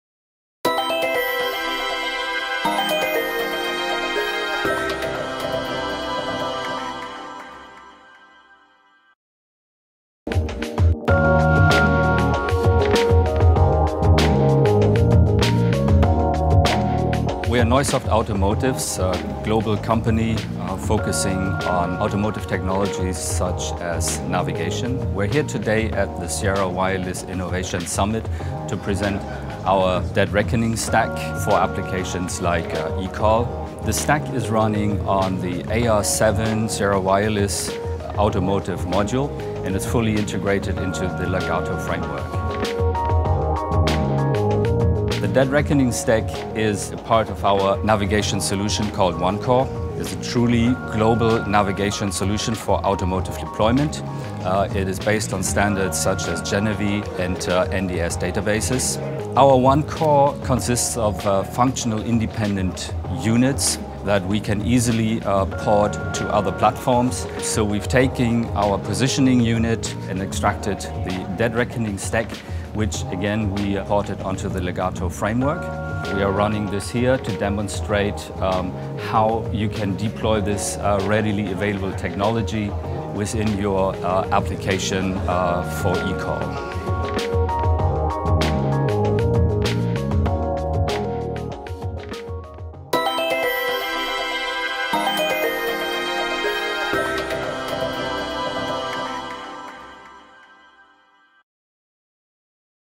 Legato Dead Reckoning implementation by Neusoft
During Sierra Wireless Innovation Summit 2015, Neusoft presented a Dead Reckoning implementation on Legato that runs on AR & WP AirPrime smart modules.

In this video, Neusoft demos it as part of their car navigation solution for global automotive deployments.